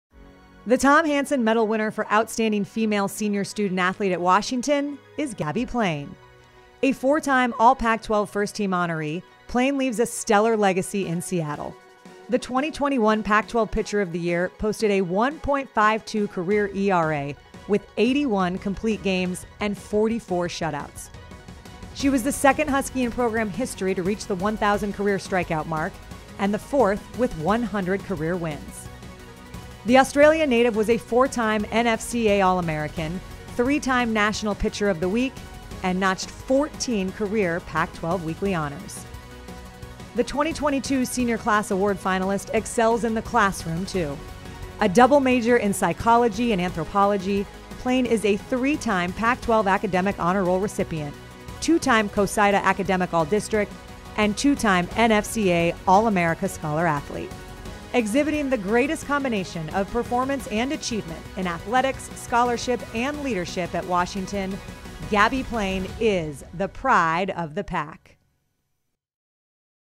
SB: Gabbie Plain is Washington's 2021-22 female Tom Hansen Medal winner
A double major in psychology and anthropology, Gabbie Plain has been named Washington's 2021-22 female Tom Hansen Medal winner. In the classroom, Plain was a three-time Pac-12 Spring Academic Honor Roll recipient (2020, 2021, 2022), a 2019 Pac-12 First Team All-Academic member, a two-time CoSIDA Academic All-District (2019, 2022) member, a third Team Academic All-American in 2022, and a three-time NFCA All-America Scholar-Athlete (2019, 2021, 2022). On the diamond, she was the 2021 Pac-12 Pitcher of the Year, a four-time First Team All-Pac-12 performer (2018, 2019, 2021, 2022), four-time NFCA All-American, and twice a First Team selection (2019, 2021). She became just the second player in Washington softball history to reach 1,000 career strikeouts and fourth with 100 career wins. She pitched for Australia in the Tokyo Olympics and currently volunteers with Rainier Athletes, a non-profit that pairs adult mentors with historically marginalized youth.